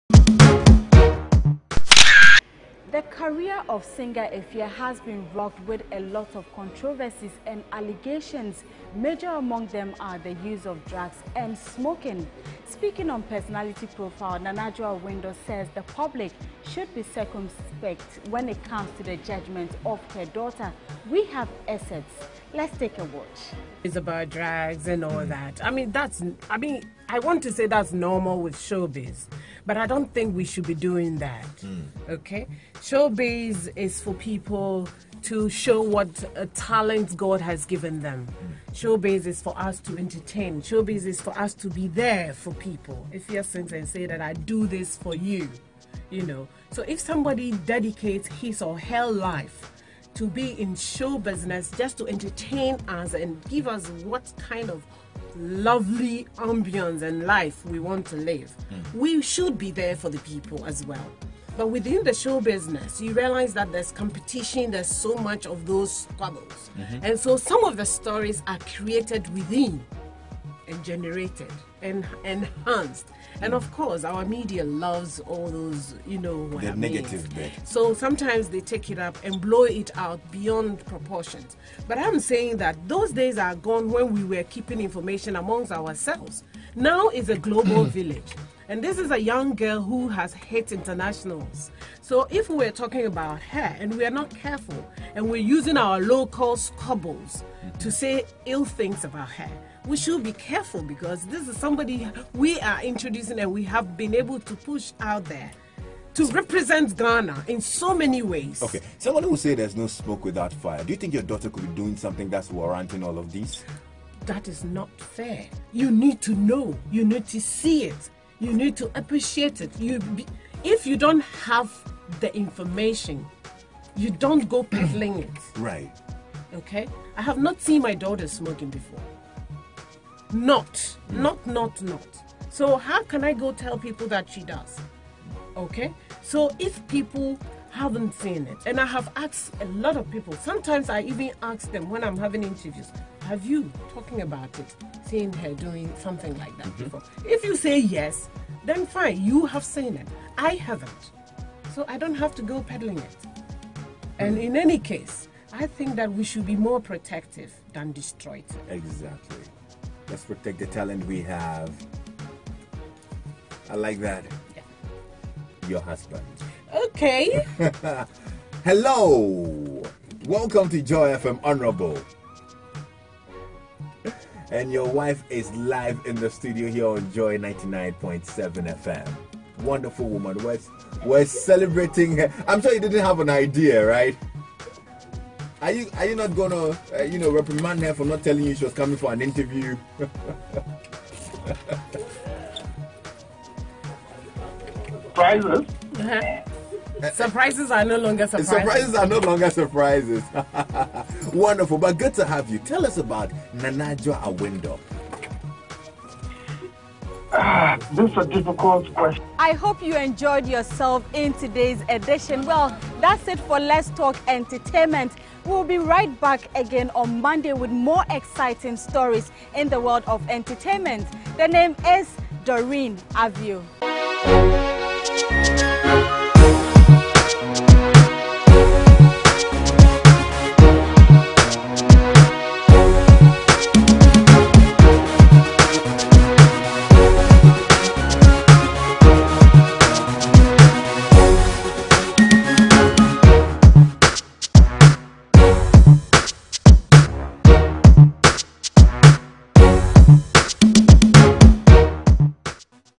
I Have Never Seen Efya Smoke - Nana Adwoa Awindor
The mother of Efya Nana Adwoa Awindor says she has never seen her daughter smoke.